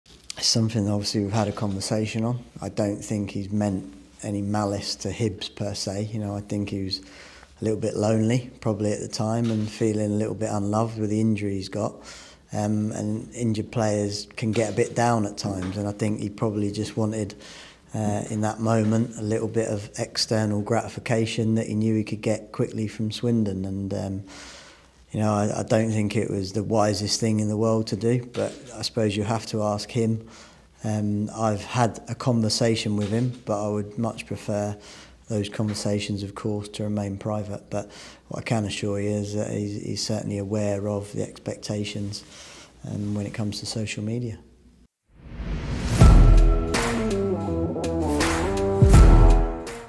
Hibs boss Johnson blames 'loneliness' for McKirdy Instagram controversy
🗣️ 'I think he was feeling a little bit lonely and feeling a little bit unloved'

Hibs boss Lee Johnson insists that forward Harry McKirdy still has a place at Hibs following Swindon Instagram controversy